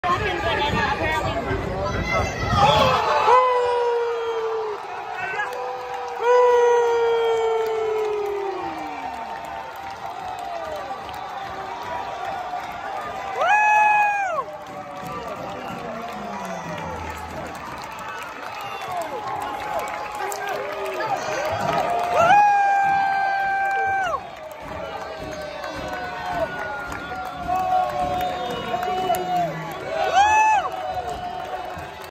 Sick Bicycle Goal: Junyor Zanelatto // Columbus Futsal
Coast to coast bicycle goal perfectly executed by Junyor Zanelatto and Gabriel Cardoso during Columbus Futsal's 2020 National Futsal Premier League home opener against MitWest FC. 12/21/19. FT CF 18x8 MWFC.